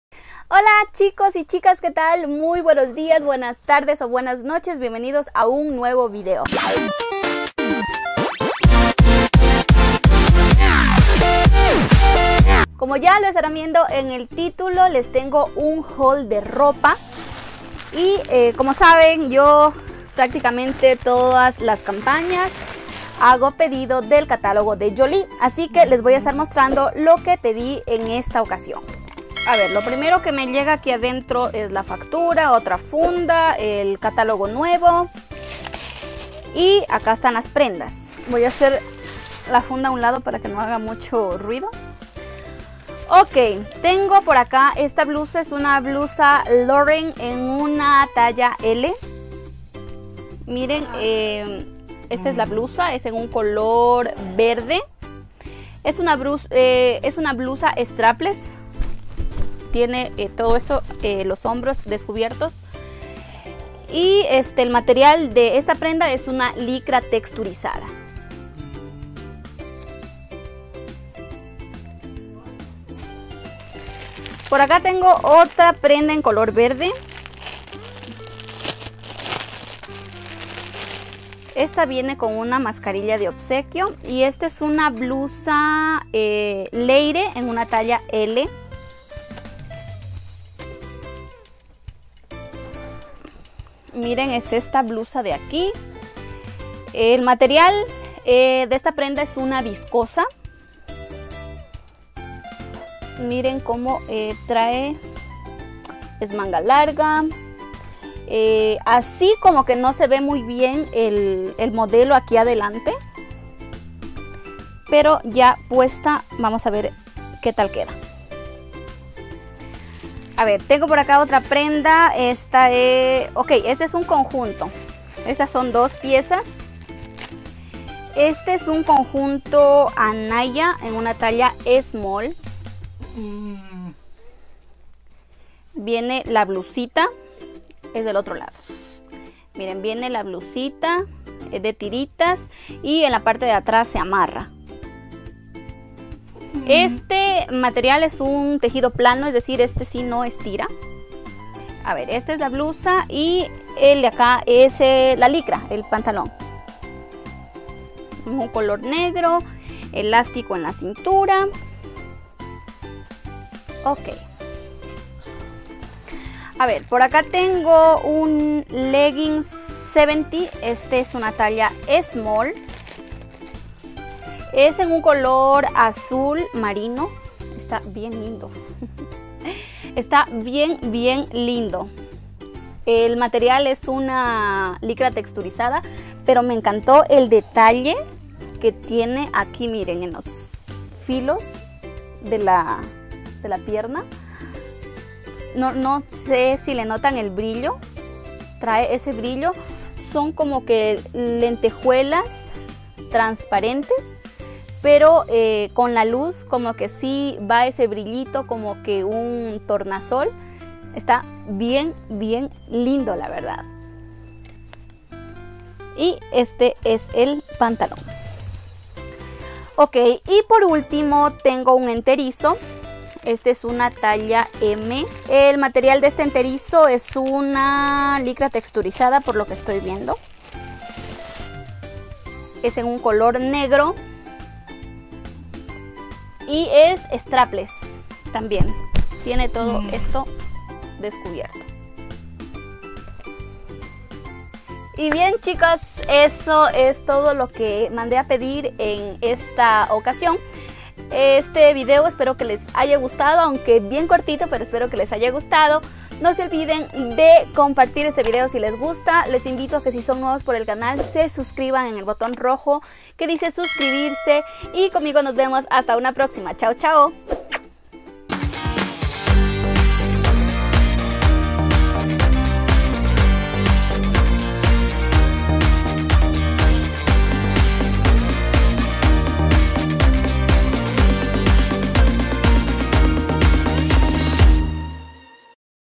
HAUL DE ROPA 👚 | Pedido del catálogo Jolie 📦 CAMPAÑA 7 - 2021 | Jeimy Ayora
Les detallo las prendas que les muestro en el video:

✅Blusa Loren, Cod. 1134111, Color: Verde, Precio: $14,50
✅Blusa Leire, Cod. 1135629, Color: Verde, Precio: $18.90
✅Conjunto Anaya, Cod. 1453708, Color: Estampado, Precio: $23,10
✅Leggins Seventy, Cod. 4033701, Color: Azul Marino, Precio: $18,30
✅Enterizo Charis, Cod. 1336405, Color: Negro, Precio: $19,60

Hola mis chicos y chicas hermos@s, bienvenidos a mi canal! Me llamo Jeimy. Aquí encontrarás de todo un poquito 👗 🧴.